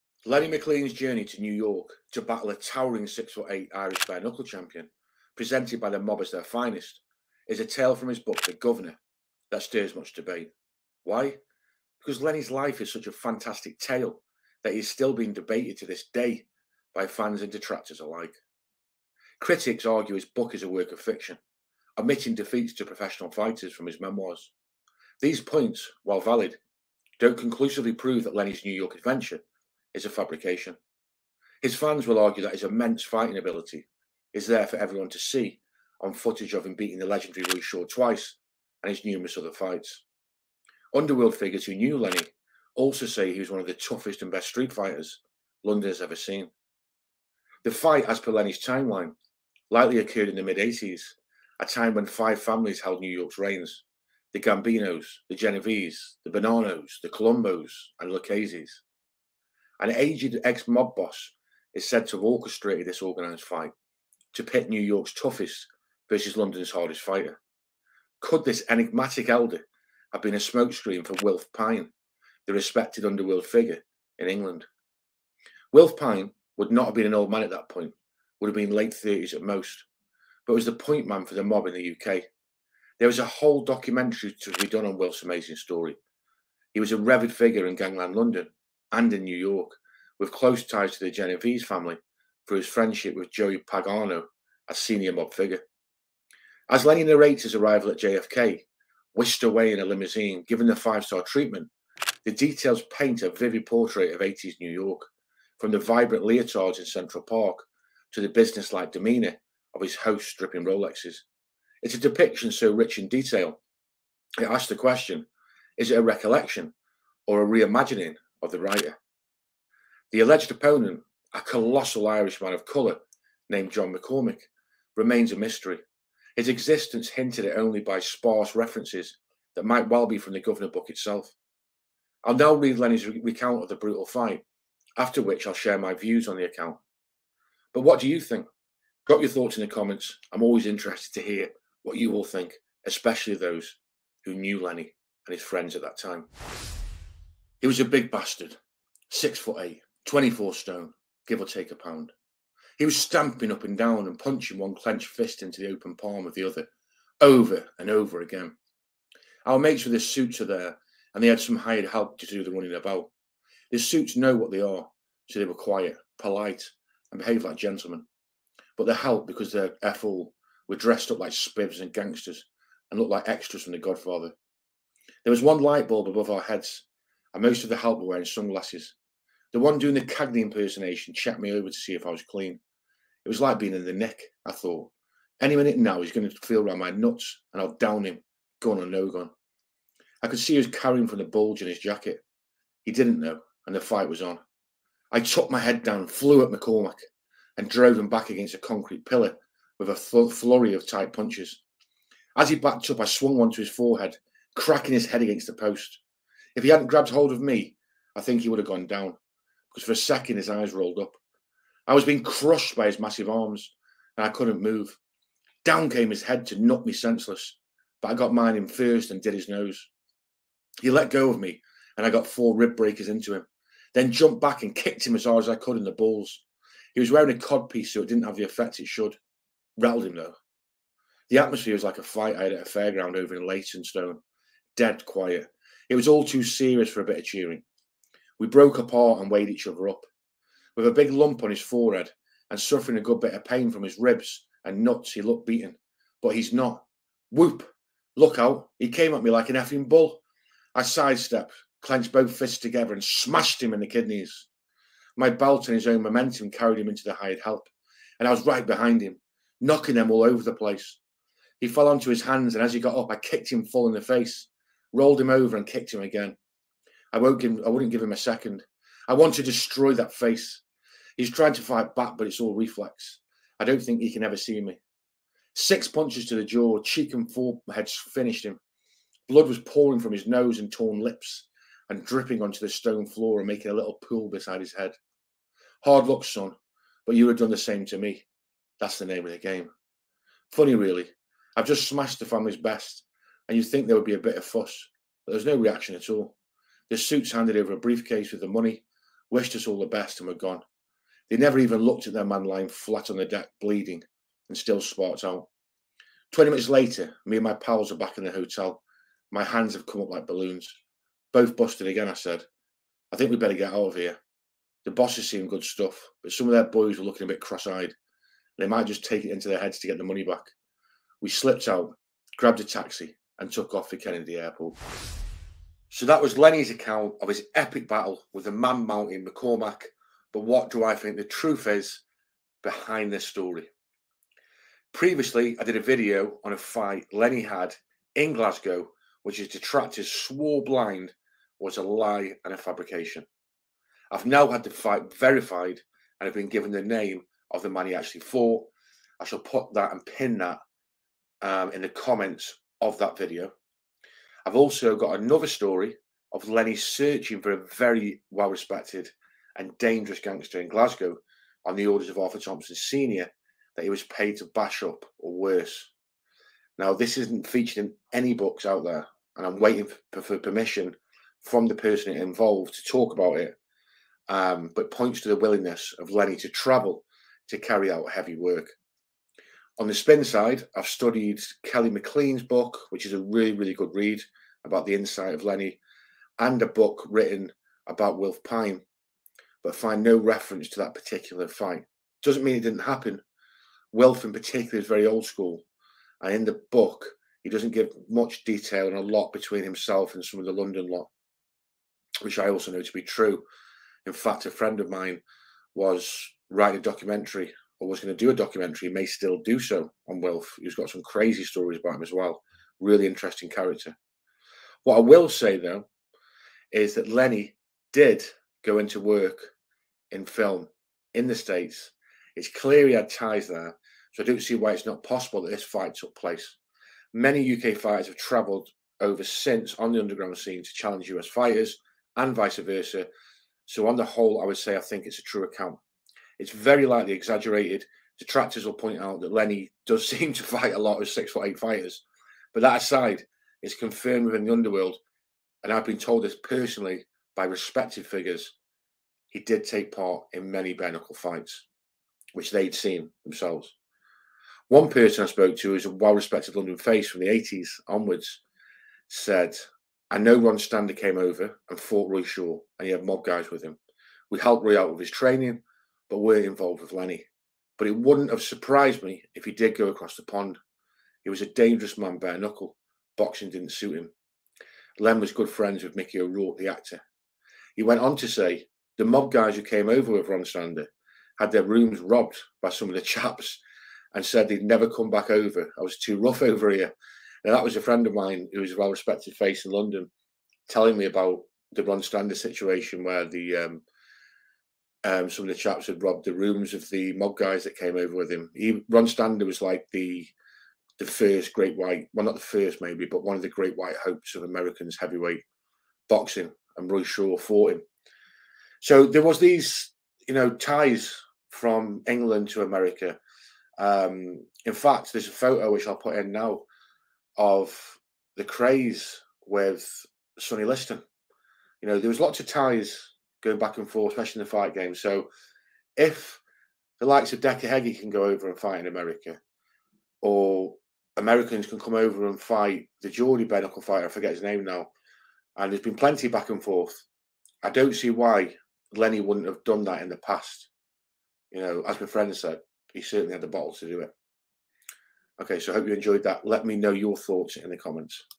Lenny McLean BRUTAL Brawl in New York
Lenny McLean faces off against a giant fighter who the New York wiseguys claim is the most fierce fighter in America and big money stakes are put up. But did it happen? I investigate and give my thoughts and read Lennie's account of the brutal fight!